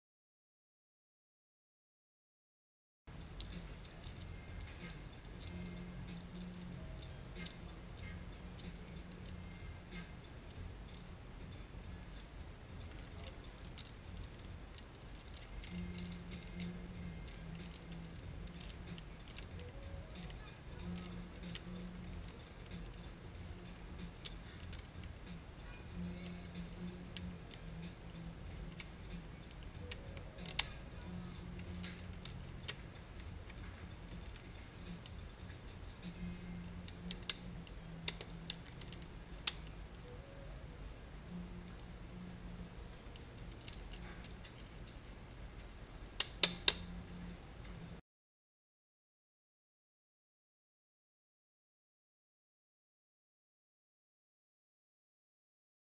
How to easily install a bike seat clamp
The basic bike seat clamp: seems simple but they can be very frustrating to install.  Using this method you should be able to install one in about 30 sec. w/ a bit of practice.